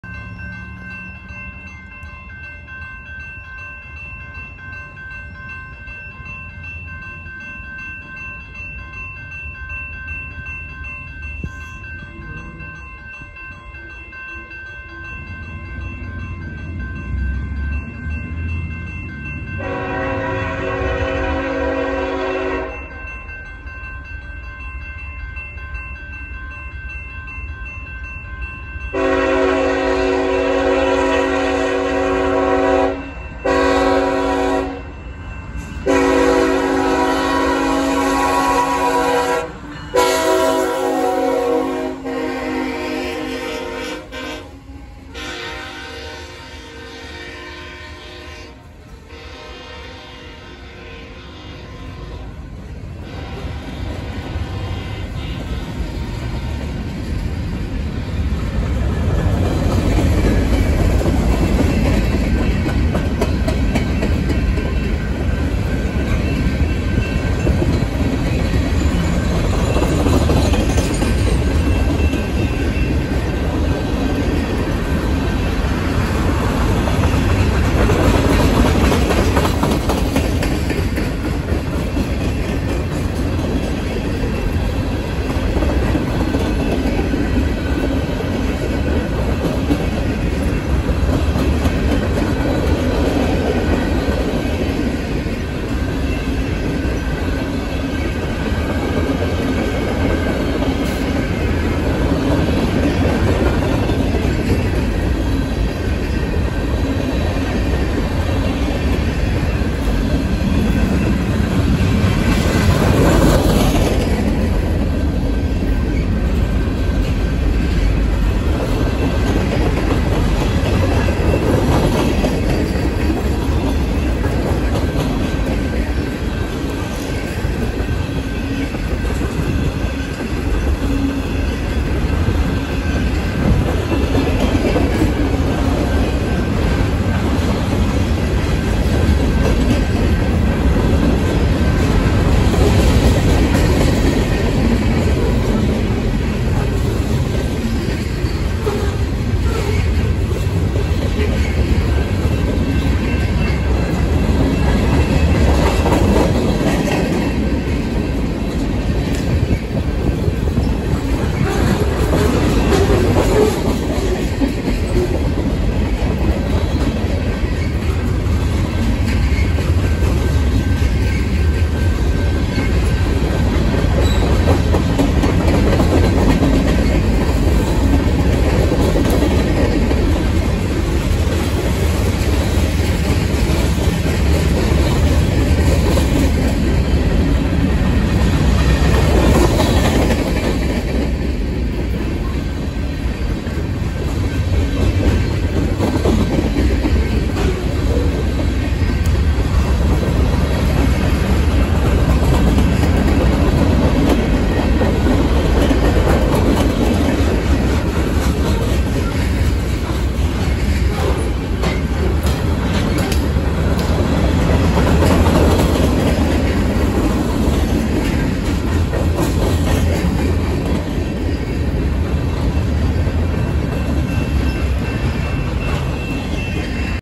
CSX eastbound manifest train passing though Plymouth
CSX L303 that’s the eastbound Grand Rapids to Detroit train with 121 cars on it headed to Rougemere Yard it is very rare to see the auto rack on this train which it has one